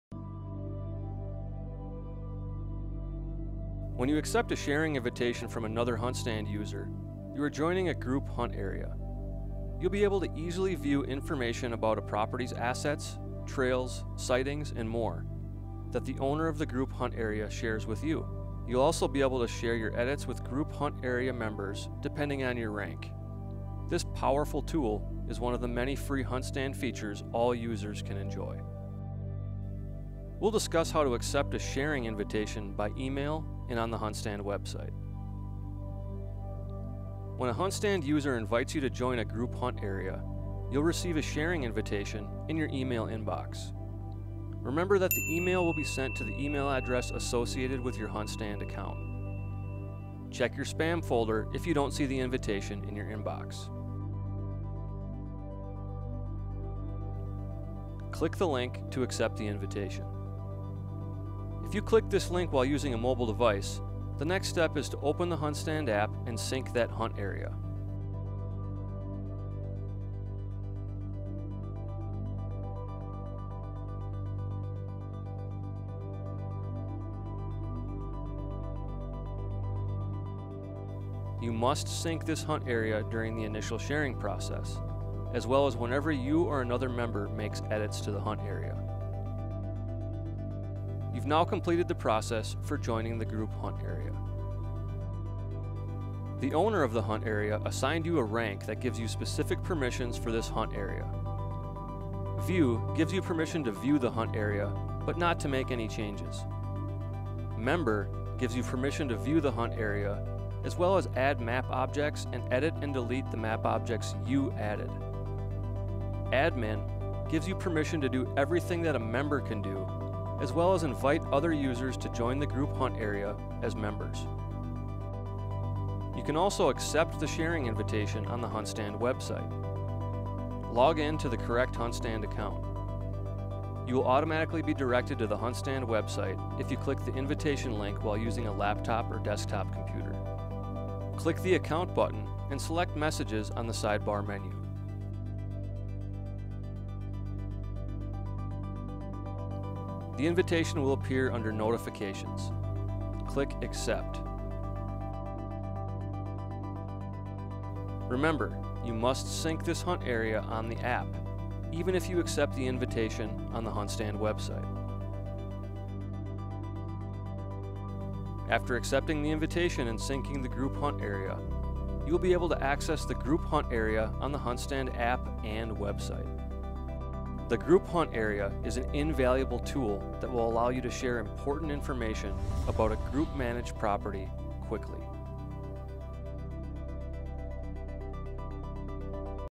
How To Accept An Invitation To Join A Group Hunt Area In The HuntStand App [Tutorial Video #10]
Learn how to accept an invitation to join a Group Hunt Area in the HuntStand app.